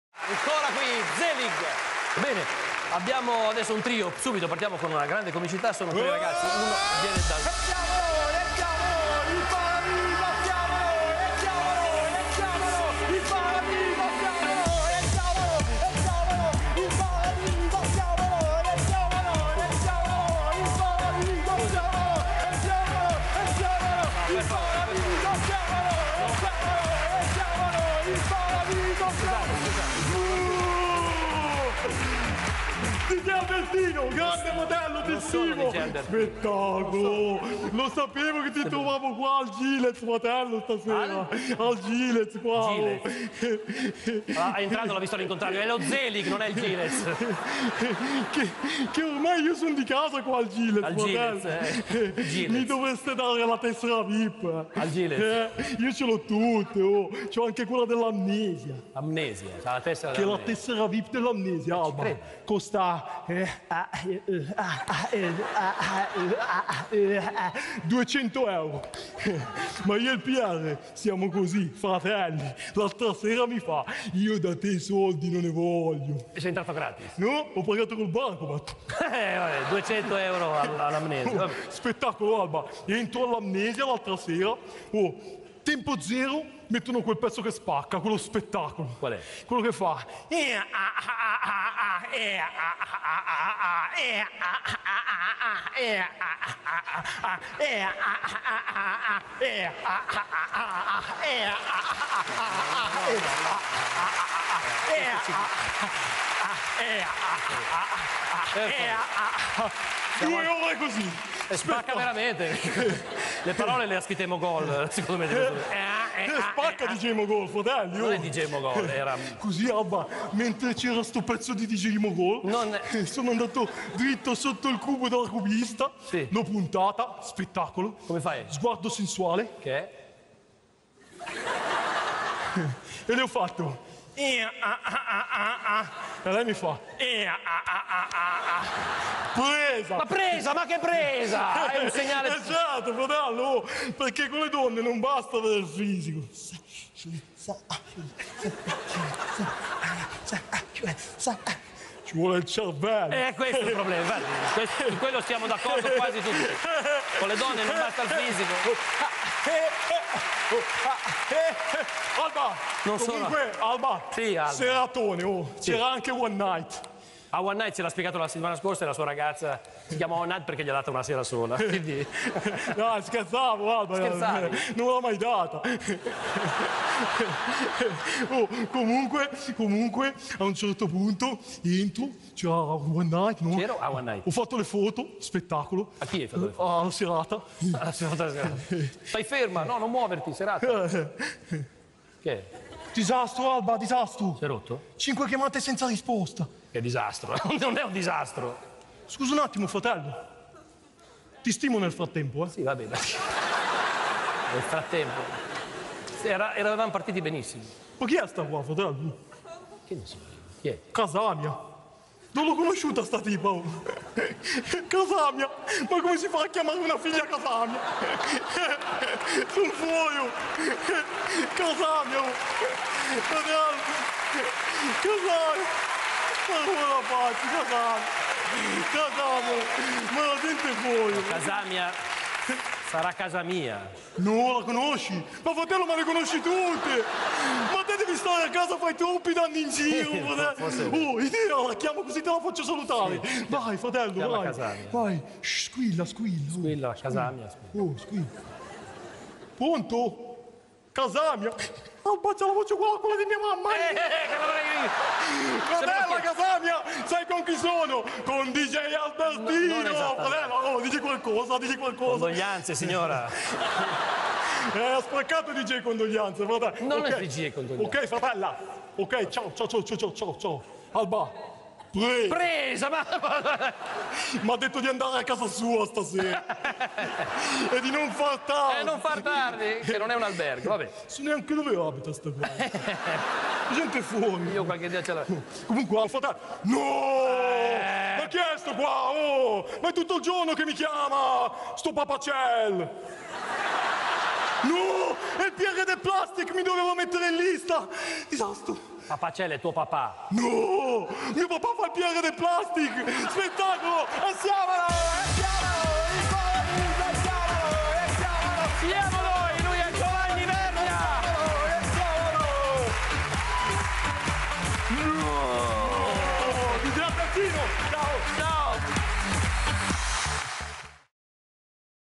GIOVANNI VERNIA/ JONNY GROOVE E "CASAMIA"
Jonny al telefono con una sedicente conquista notturna, tal "Casàmia".